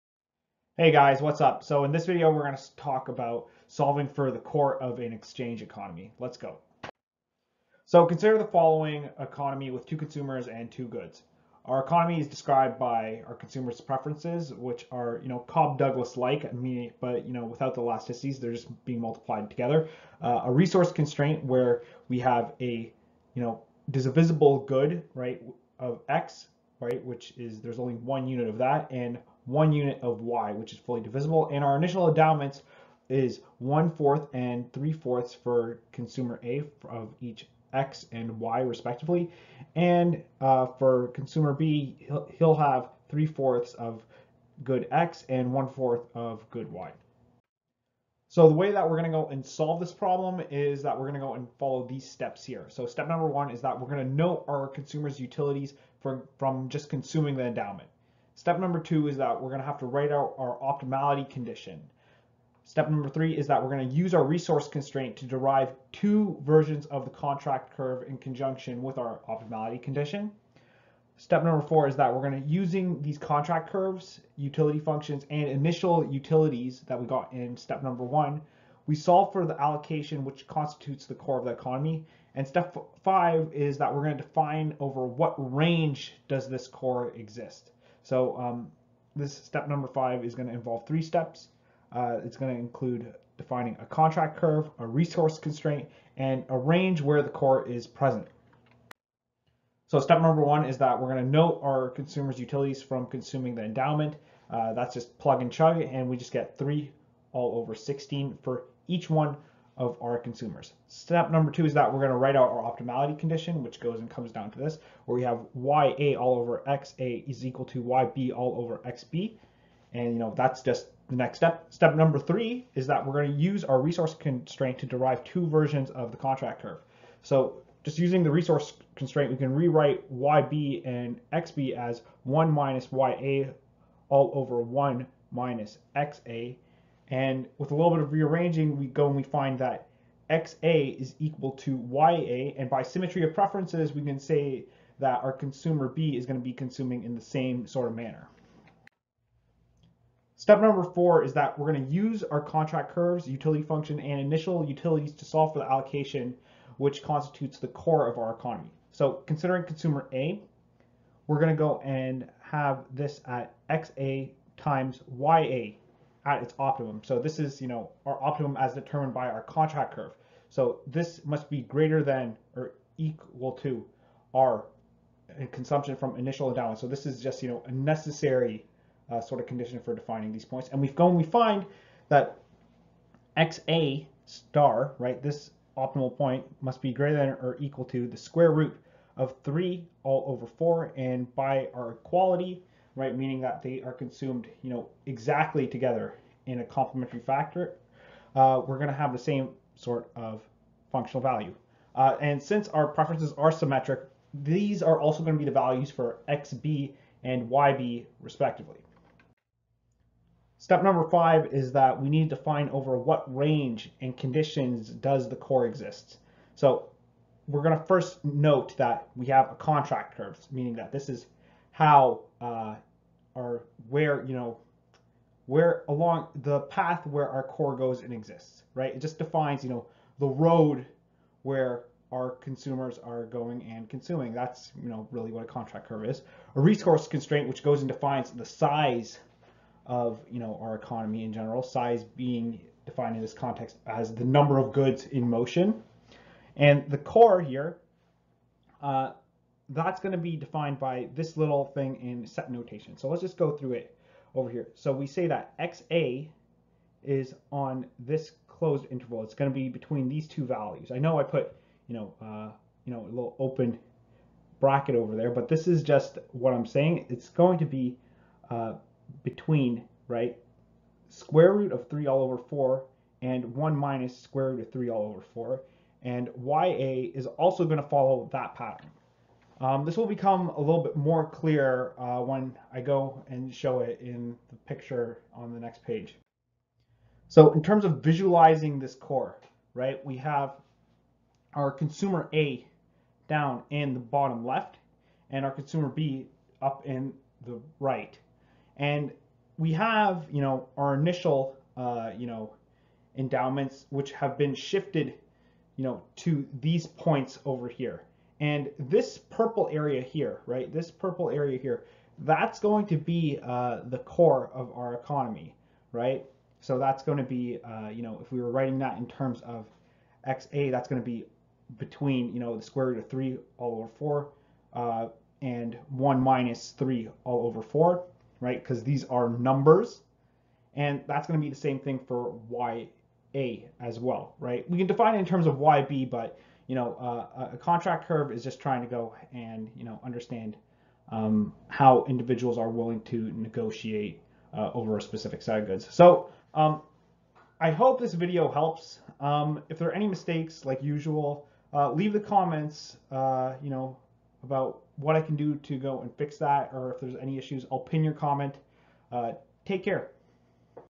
Exchange Economies: Core Practice (Example #1)
In this video we go through an example of how to solve for the core of an exchange economy.